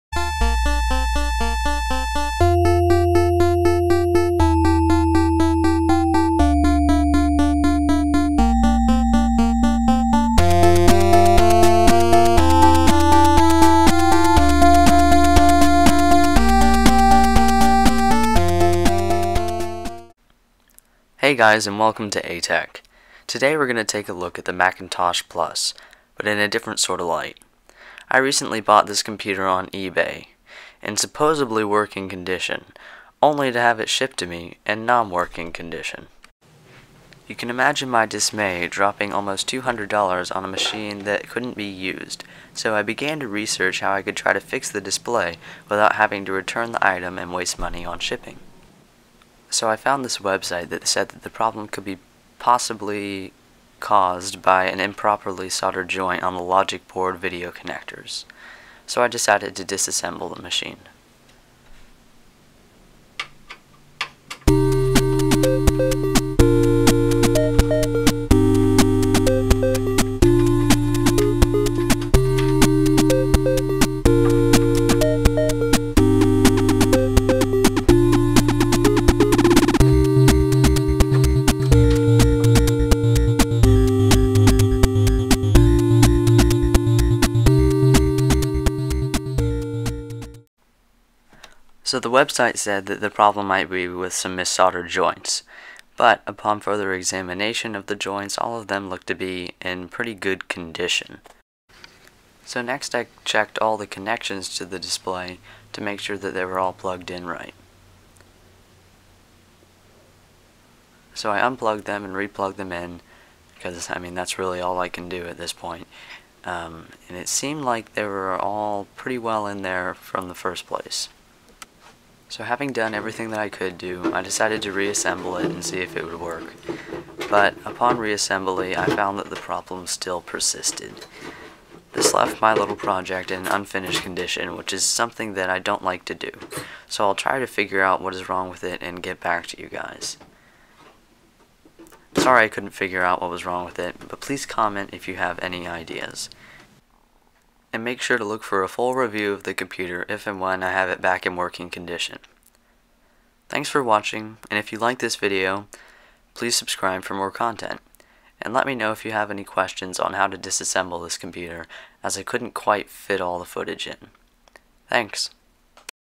Macintosh Plus Fix
I try to fix a display problem with my Macintosh Plus.